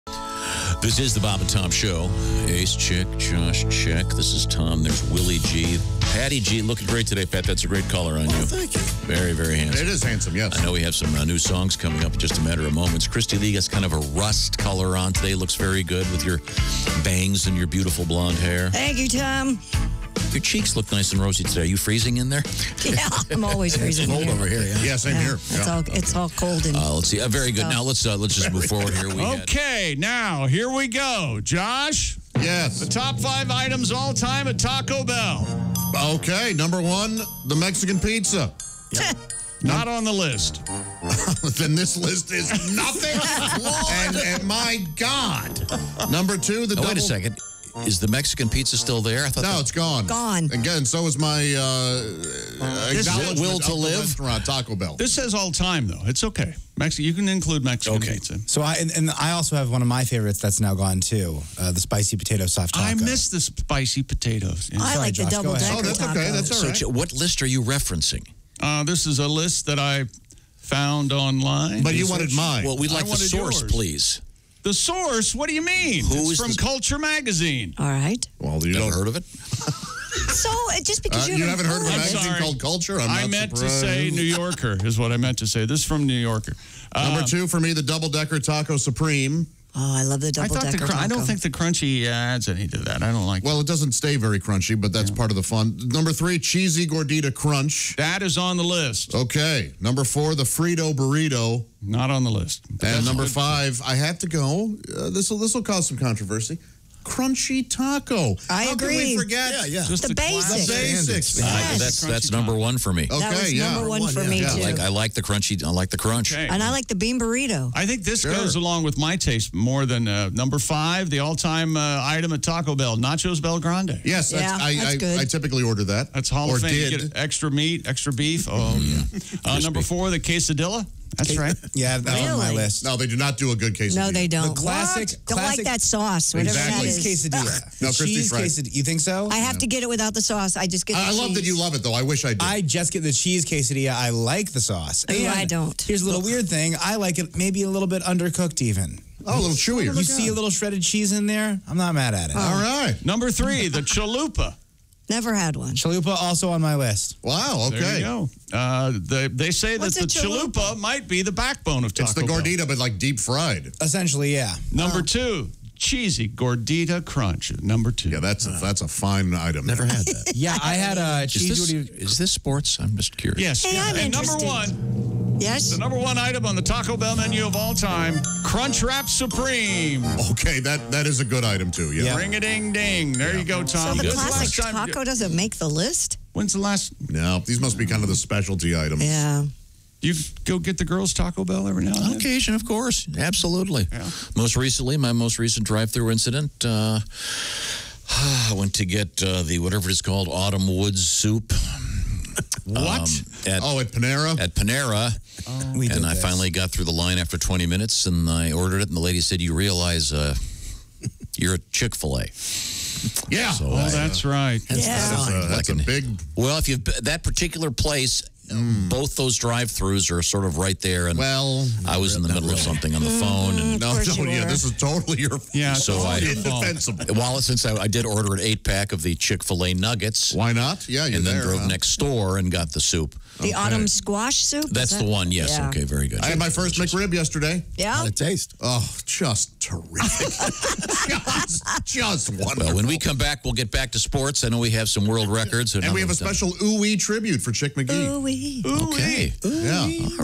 Josh Arnold's Top Five Taco Bell Items
Josh Arnold is a known eater and is a big fan of Taco Bell. He leads the gang in listing their top five items.